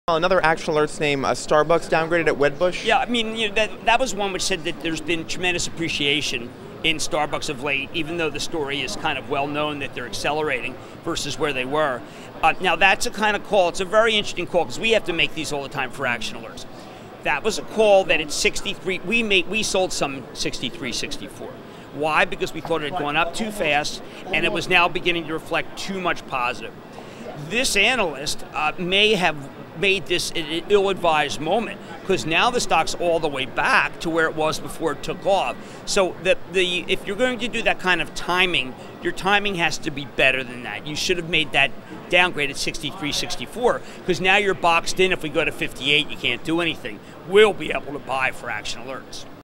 Wedbush's Downgrade of Starbucks May Have Come at an Ill-Advised Moment, Jim Cramer Says
TheStreet's Action Alerts PLUS Portfolio Manager Jim Cramer questions the timing of Wedbush's downgrade of Starbucks SBUX on Wednesday.

The stock is a holding of Action Alerts PLUS.